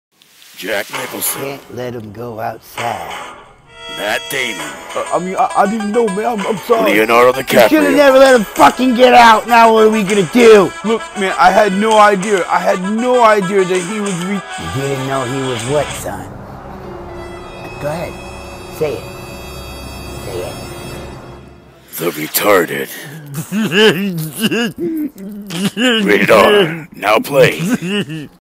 movievoicesmakeanythingsoundawesome : the RETARTED
Just jokes, it's a play off of the departed, so don't get your panties up in a bunch